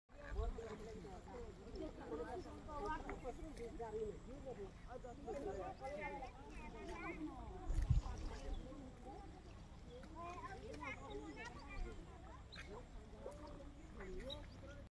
white herons
white herons in my paddy fields feeding on earthworm & other insects